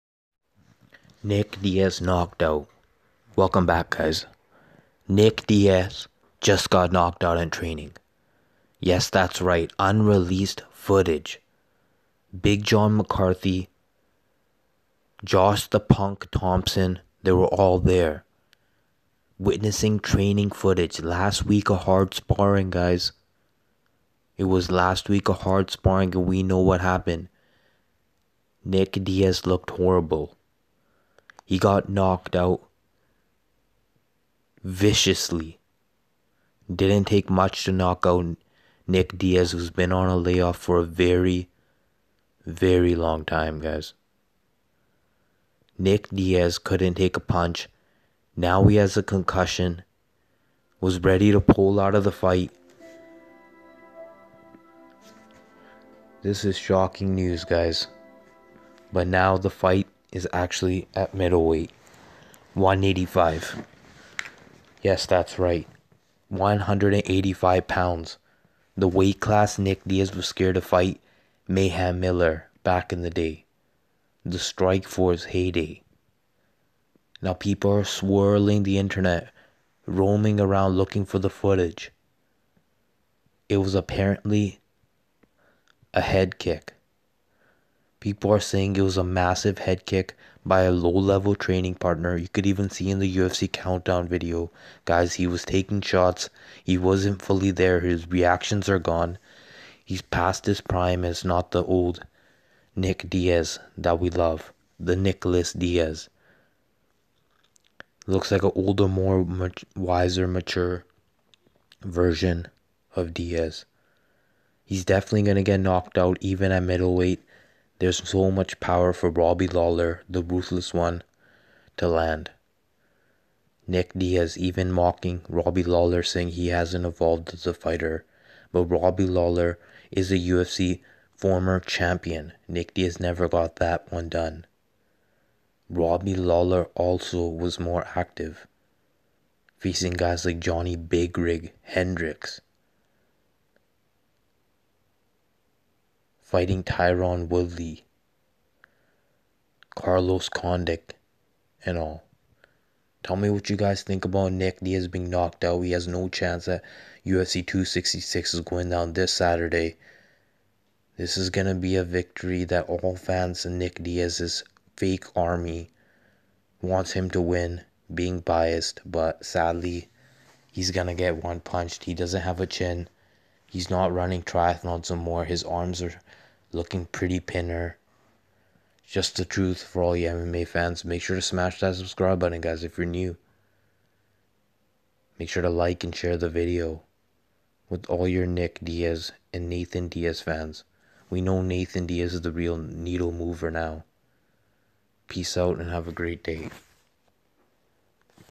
UNSEEN NICK DIAZ KNOCKED OUT COLD BEFORE UFC 266
He is going to get one punched. Robbie by massive KO.